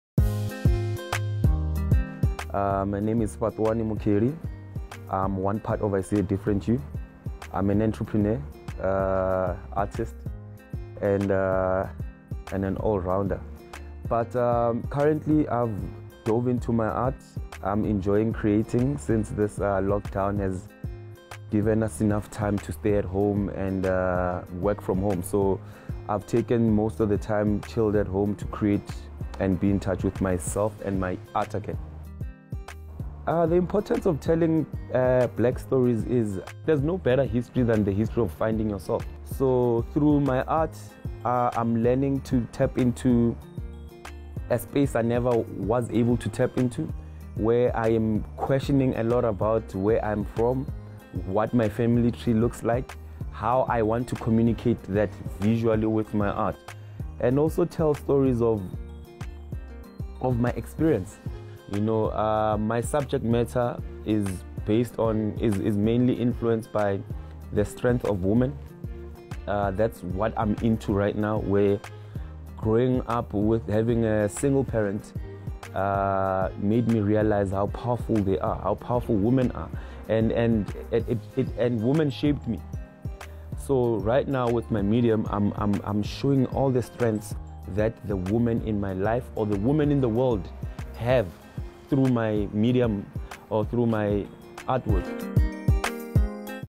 Fhatuwani Mukheli on using art to tell black stories.
Co-founder and director of creative agency I See A Different You Fhatuwani Mukheli speaks about how he utilizes art to tell the stories of black people from past and present.